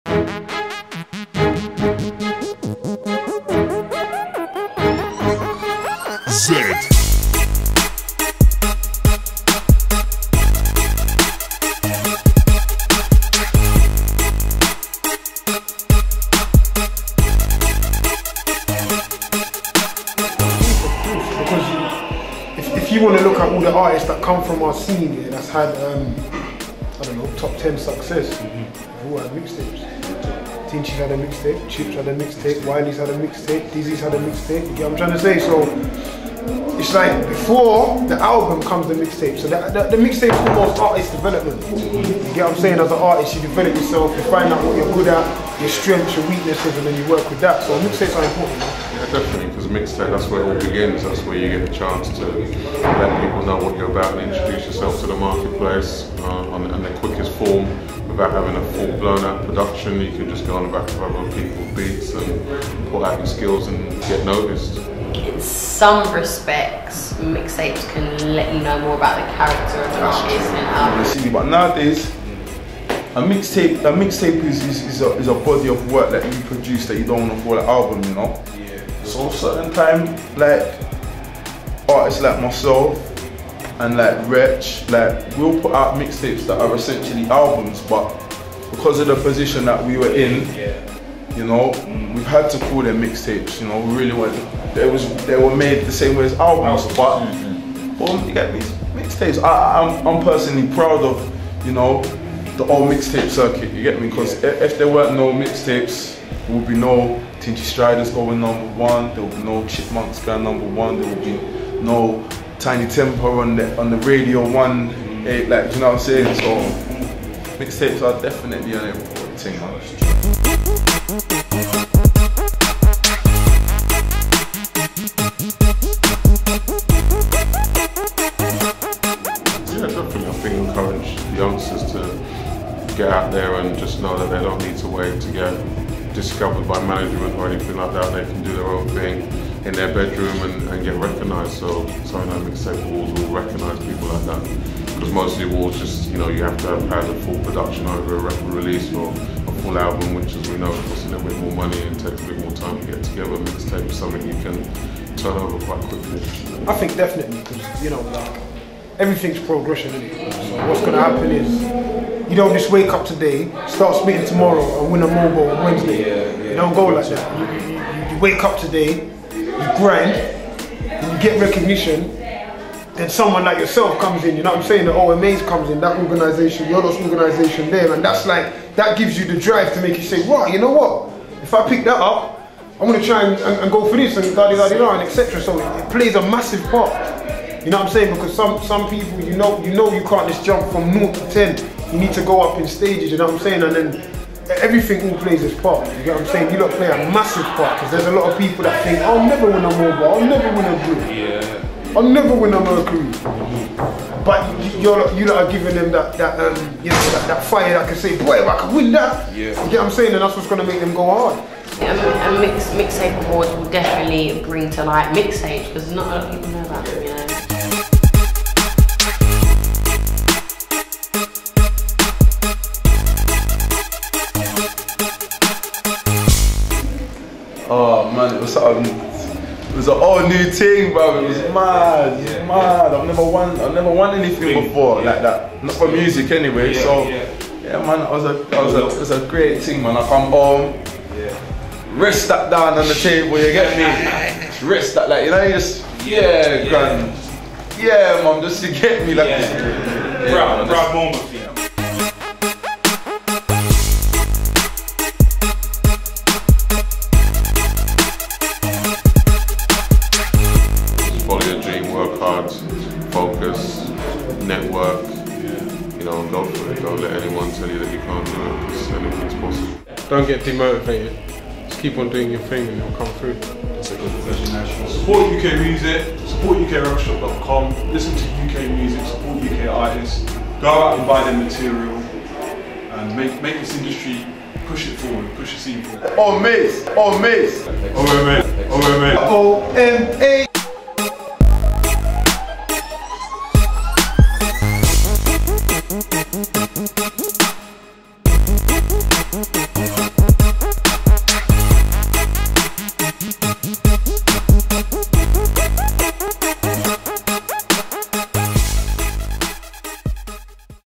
CVTV: OMA, Wretch 32,Tulisa, Schorcher, Chris Nethanial
OMA Interviews some influential and powerful individuals in this video.
Filmed by: Andre Crawford
More Videos at CrystalVisuals.com
Follow: @CrystalVisuals